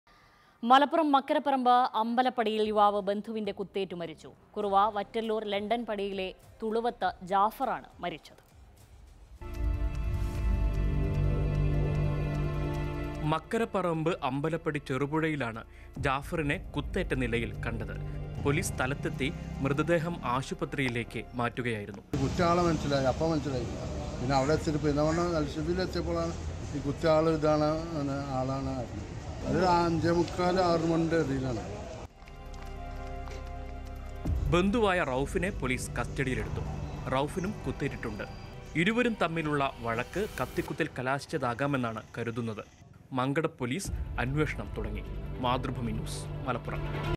വഴക്ക് കത്തിക്കുത്തിൽ തീർത്തു; മലപ്പുറത്ത് യുവാവ് ബന്ധുവിൻ്റെ കുത്തേറ്റ് മരിച്ചു | Crime |Malappuram
മലപ്പുറം മക്കരപ്പറമ്പ് അമ്പലപ്പടിയിൽ യുവാവ് ബന്ധുവിൻ്റെ  കുത്തേറ്റ് മരിച്ചു. കുറുവ വറ്റല്ലൂർ ലണ്ടൻ പടിയിലെ തുളുവത്ത്  ജാഫറാണ് മരിച്ചത്.

Happy viewing!